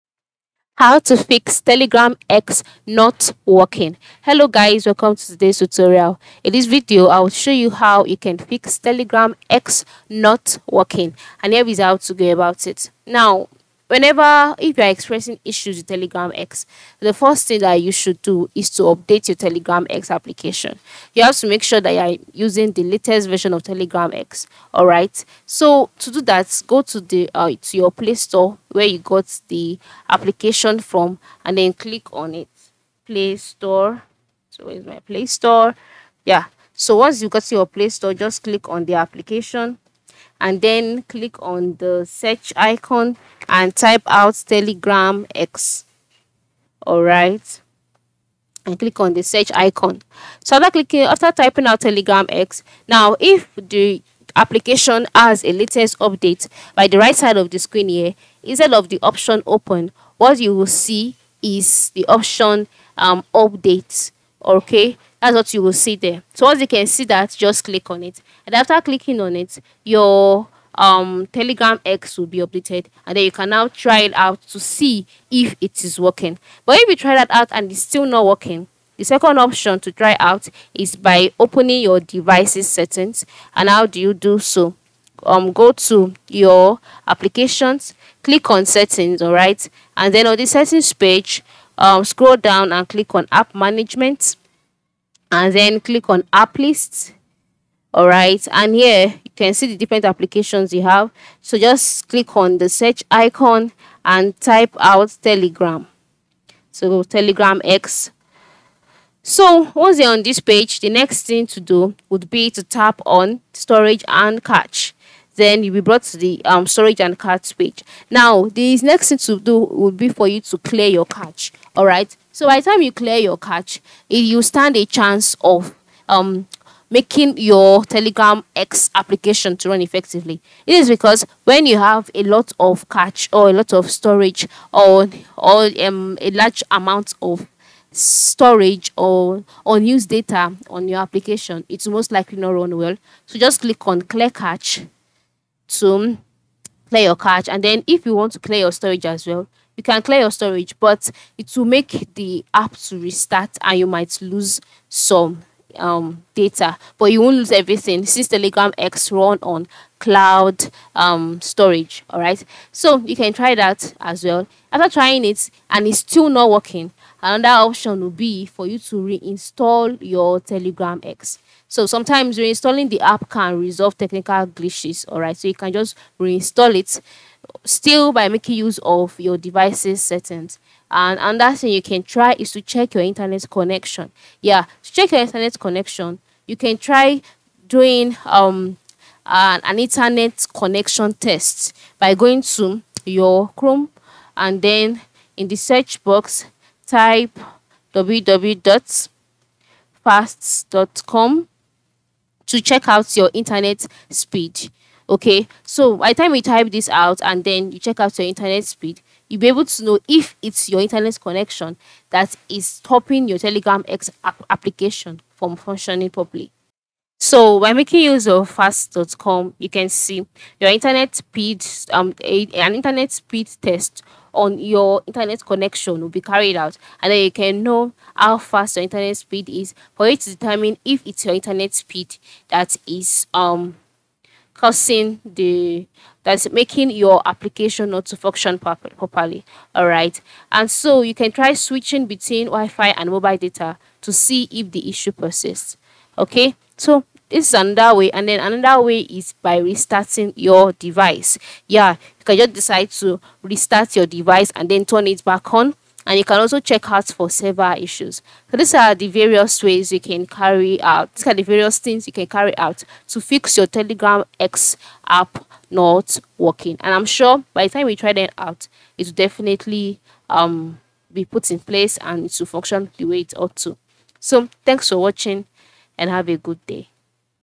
How To Fix Telegram X Not Working (Quick Tutorial)
In this video I will show you How To Fix Telegram X Not Working. It would be good if you watch the video until the end so that you don't miss important steps.